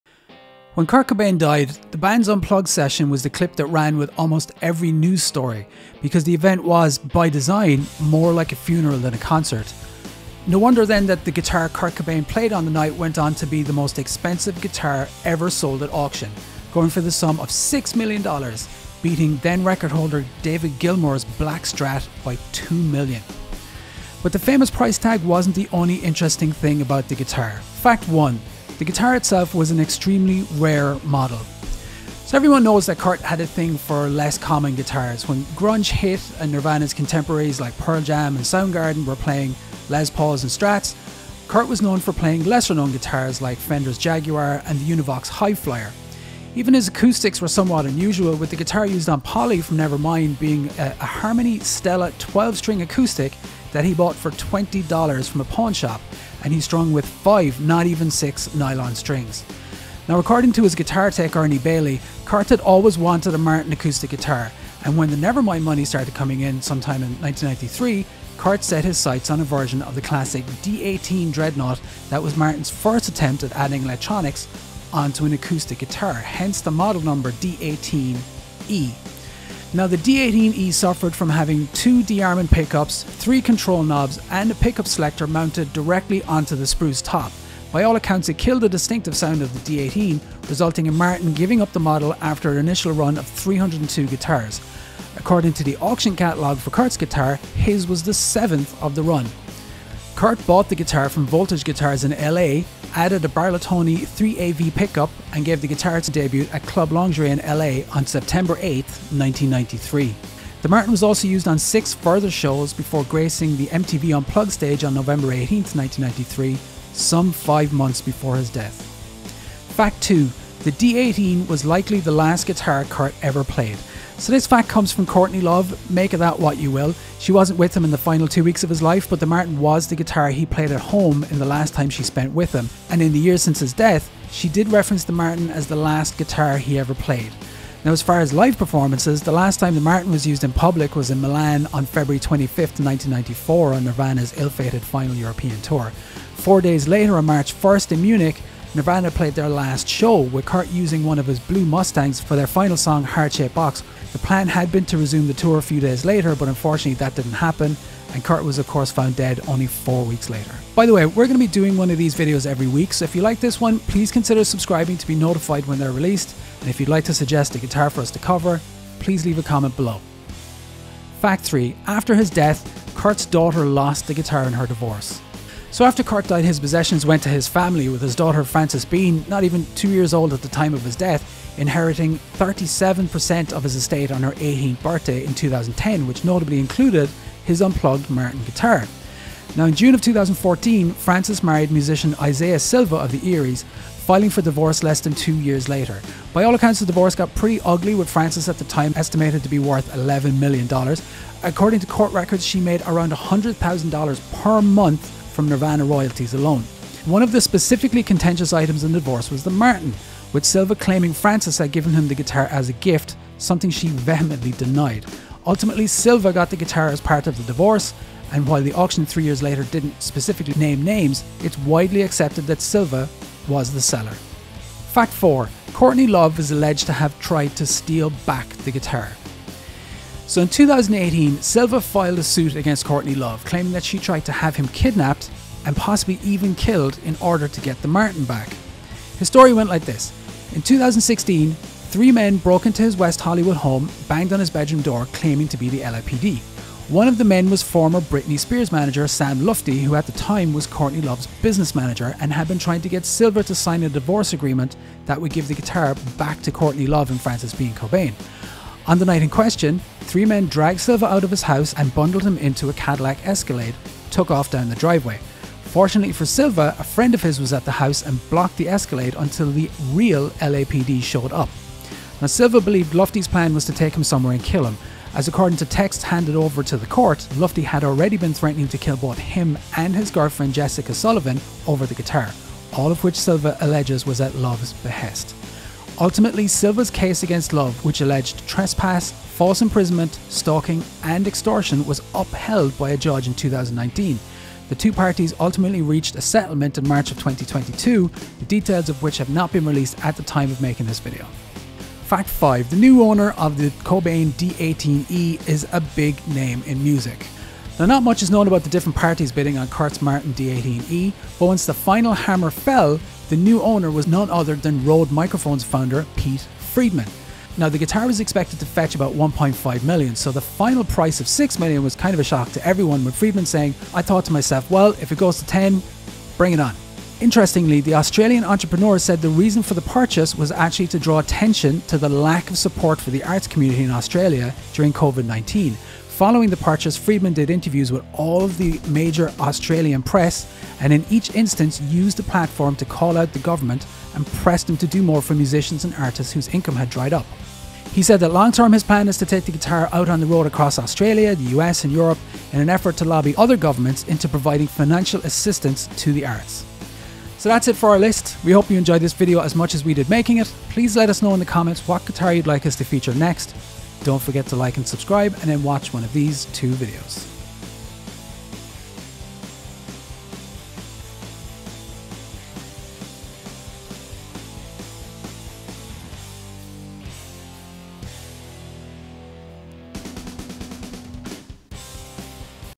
Crazy Stories Behind Kurt Cobain's Nirvana Unplugged Guitar (Martin D-18e)
Today we're looking at 5 facts about Kurt Cobain's Martin D-18e - the guitar he used on Nirvana's MTV Unplugged performance, and the most expensive guitar ever sold at auction.

0:00 Intro
0:27 The Martin Is An Extremely Rare Model
1:59 The Guitar Might Be The Last Kurt Ever Played
2:50 Frances Bean Cobain Lost The Guitar In Her Divorce
3:44 Courtney Love Allegedly Tried To Steal The Guitar Back
5:01 The New Owner Is A Big Name In Music